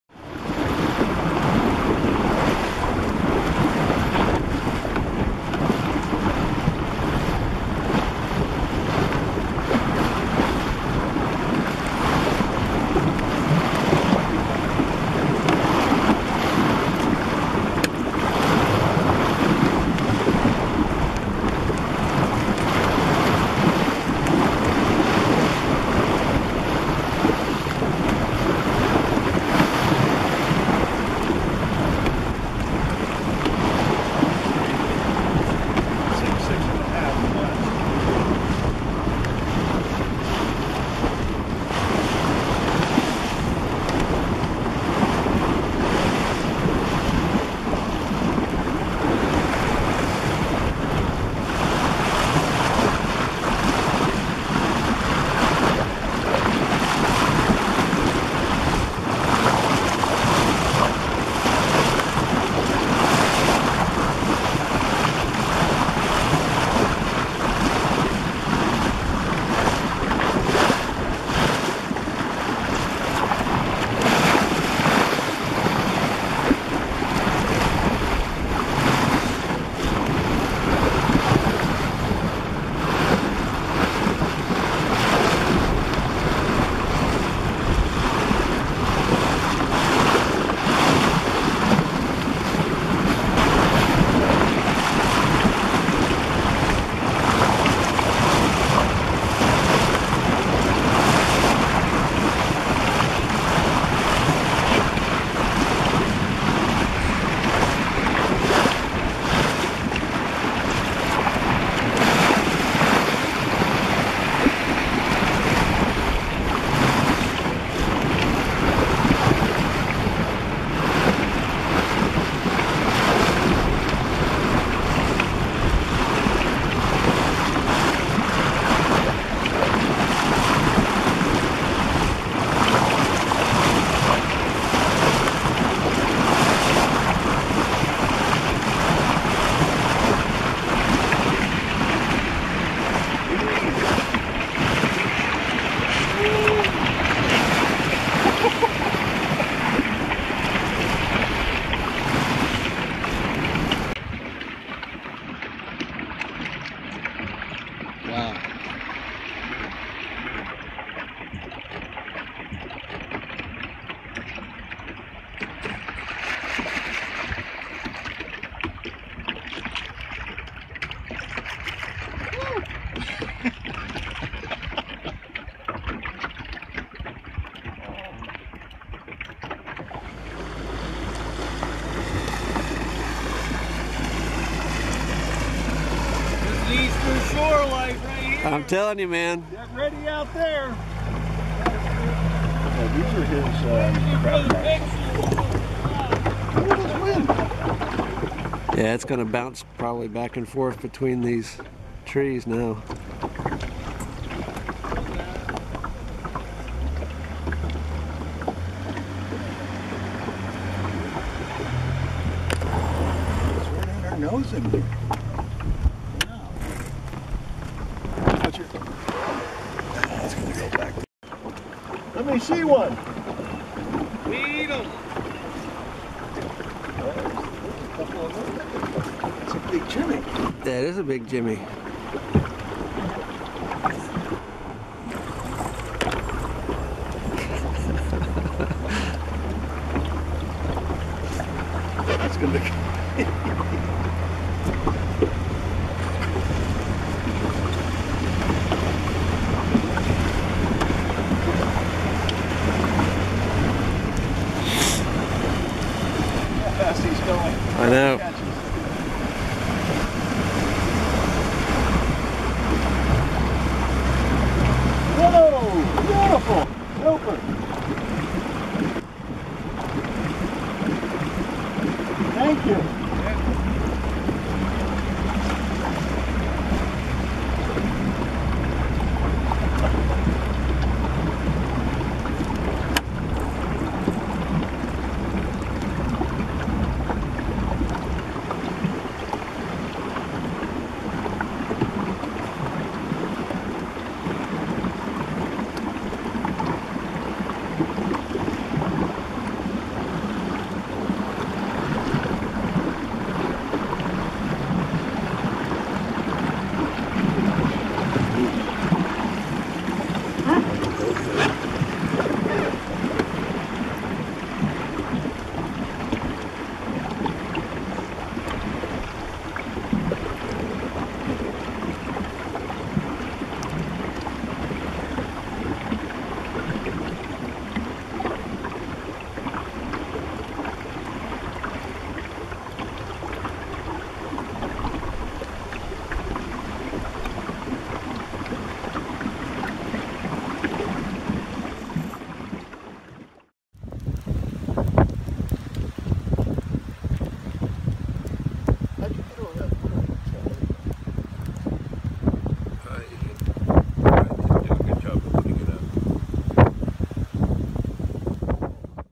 Big Downwind Sleigh Ride
Sailing downwind in a Caledonia Yawl in about 20 knots of wind near Crisfield, on the Eastern Shore of Maryland. With crabs!